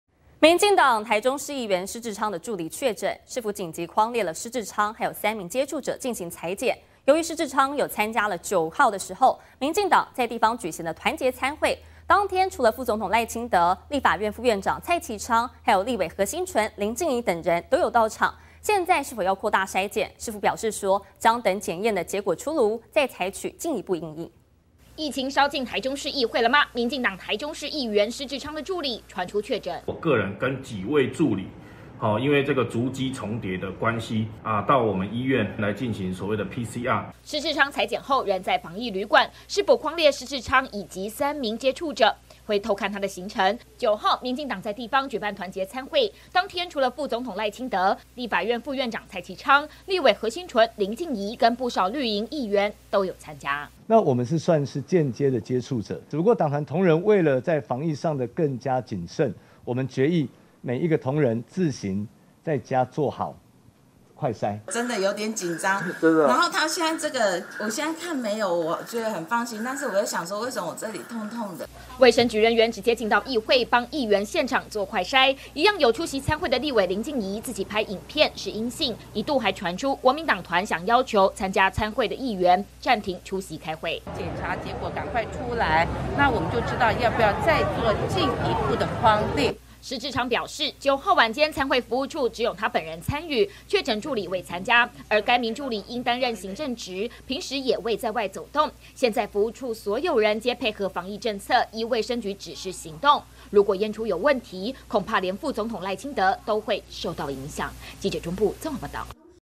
地方團結餐會 副總統.立院副院長皆參加
民進黨台中市議員施志昌的助理確診，市府緊急匡列施志昌以及3名接觸者進行採檢，由於施志昌有參加9號民進黨在地方舉辦團結餐會，當天除了副總統賴清德，立法院副院長蔡其昌、立委何欣純、林靜儀跟多名議員都到場，現在是否要擴大篩檢，市府表示將等檢驗結果出爐，再採取進一步因應！